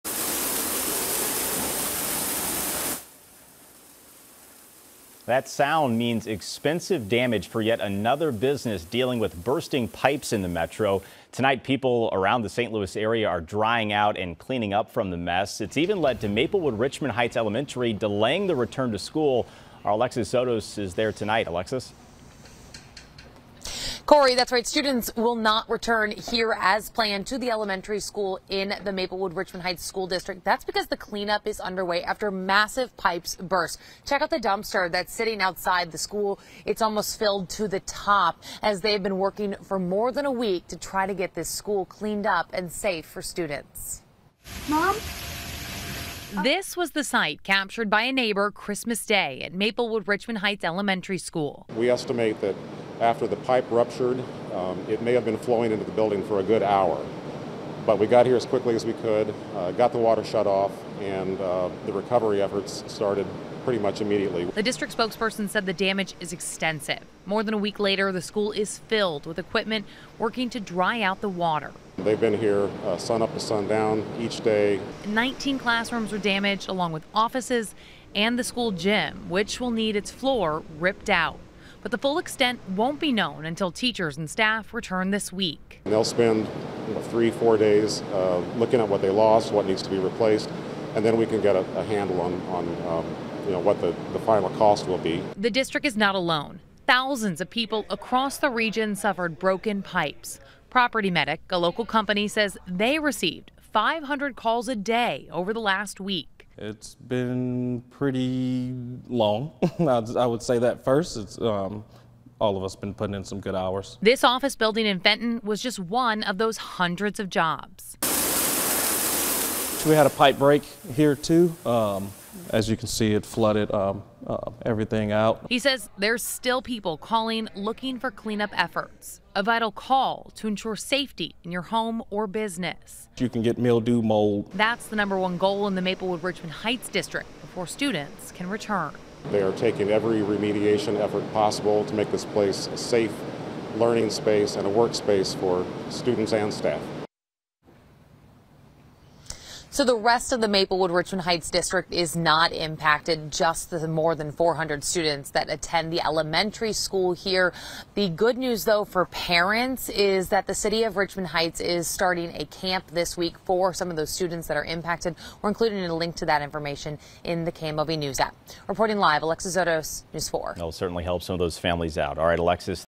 Maplewood Richmond Heights Elementary School to remain closed following massive water pipe ruptur...
Maplewood Richmond Heights Elementary School to remain closed following massive water pipe rupture